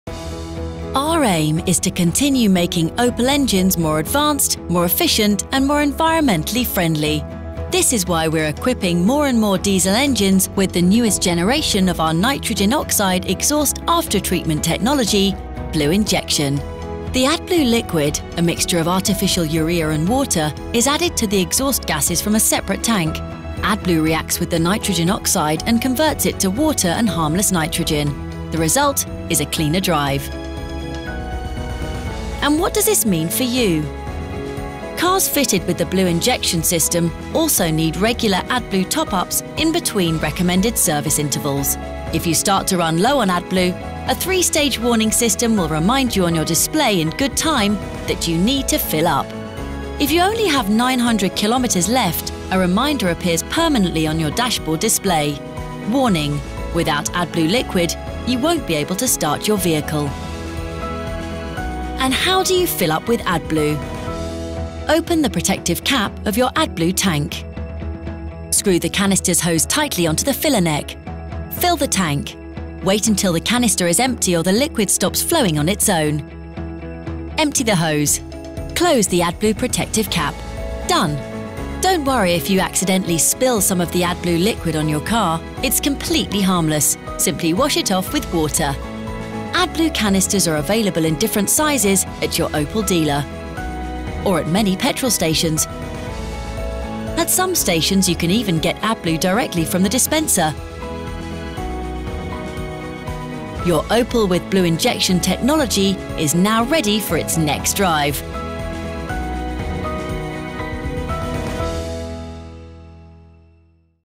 OPEL presents: BlueInjection Technology
BlueInjection is the cutting-edge SCR diesel technology from Opel that uses AdBlue®* to further reduce the level of nitric oxide and work towards cleaner emissions.

By owning a vehicle with BlueInjection technology you are helping to reduce harmful mono-nitrogen oxide (NOx) emissions thus reducing your environmental footprint. Learn how easy it is to fill up your AdBlue®* tank.

*AdBlue® is a registered trademark from Verband der Automobilindustrie e.V.